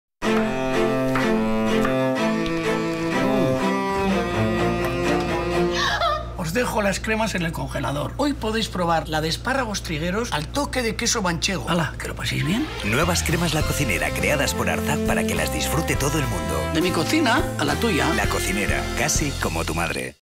La Cocinera. Arzak
Campaña 2012 Cremas La Cocinera con Arzak.